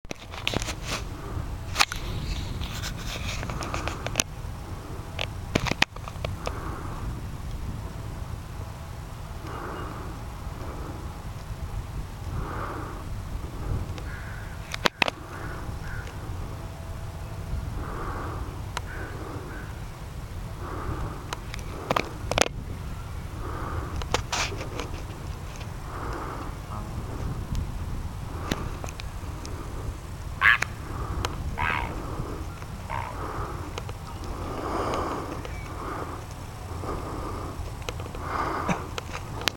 Great Blue Heron takes flight
Poorly focused shot of a Great Blue Heron  taking off from the Oasis at Wreck Beach